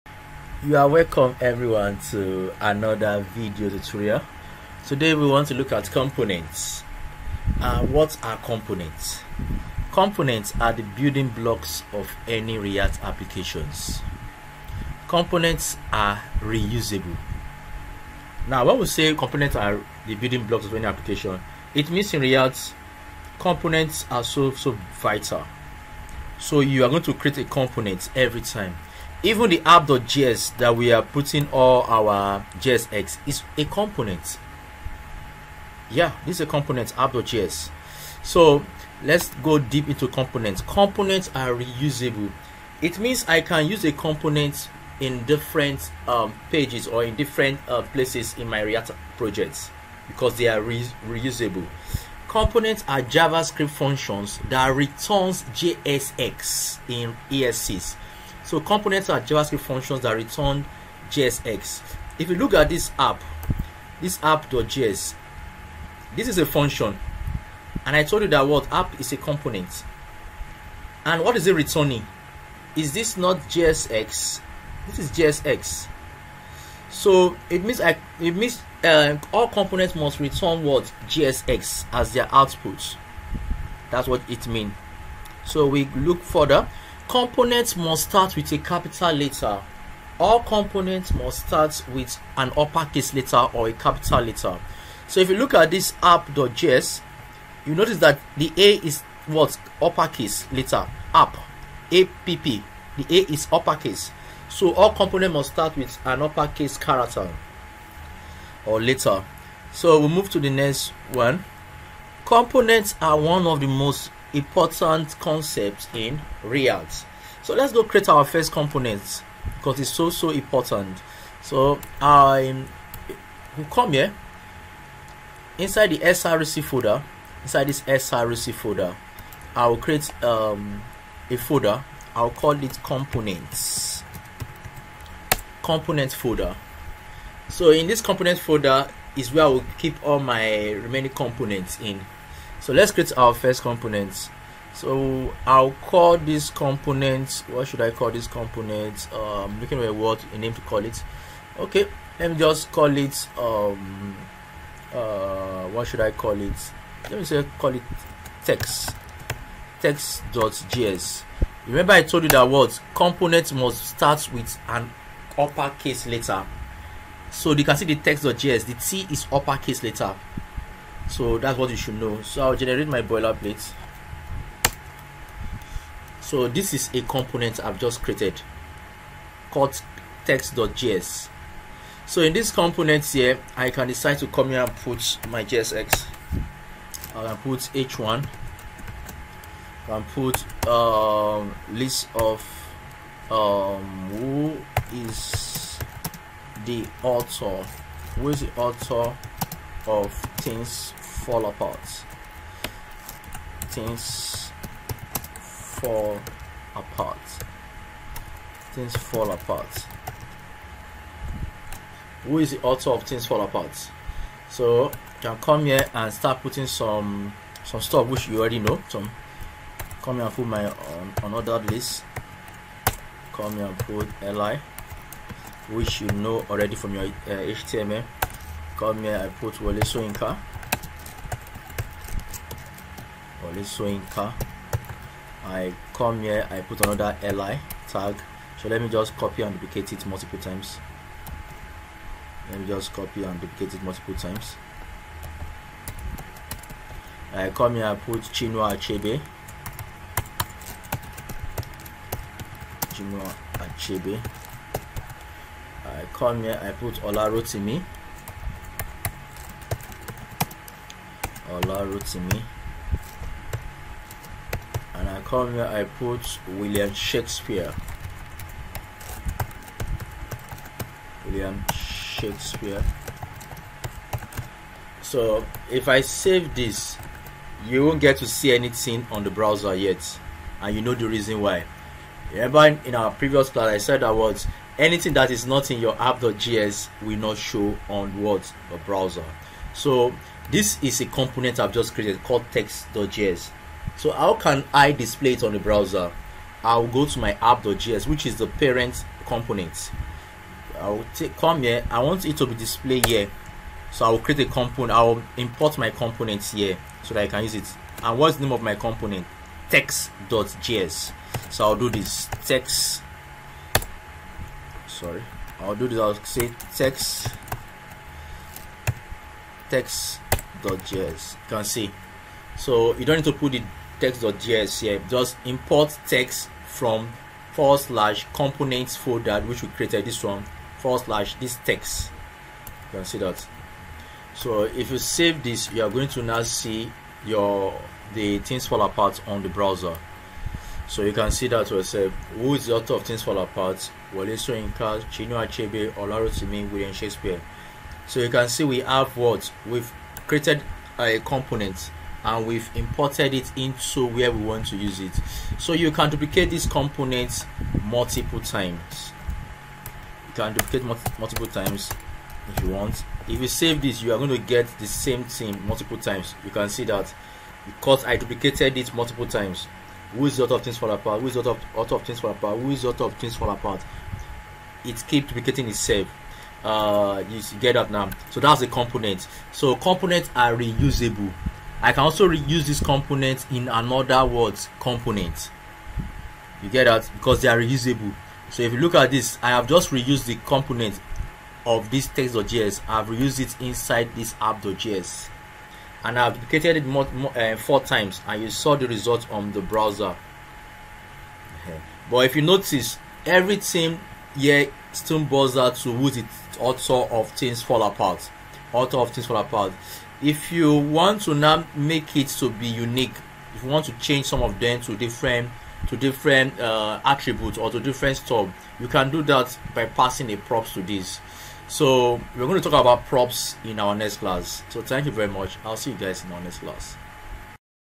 7. Components in React
This video will teach you about React component and their re-usability features.